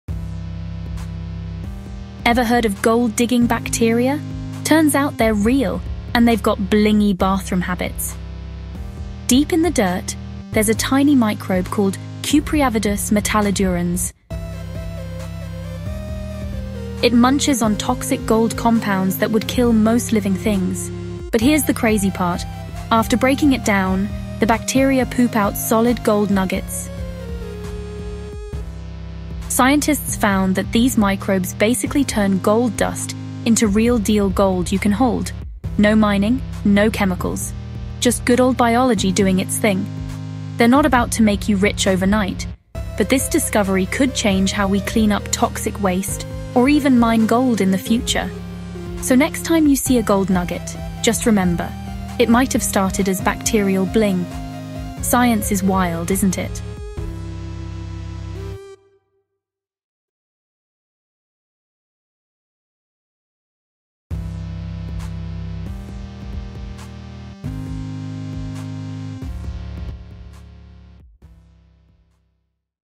“Wait… Bacteria Poop GOLD?! 😳
Yes, there is a bacteria called Cupriavidus metallidurans that converts toxic heavy metals, including gold compounds, into pure 24-karat gold nuggets through a detoxification process. This microbe survives in extreme, toxic environments, such as contaminated soils in mines, by using enzymes to neutralize lethal gold ions and precipitate them as solid gold particles that it excretes as waste. This process, known as biomineralization, provides an eco-friendly, biological pathway for gold recovery and helps shape mineral deposits in nature.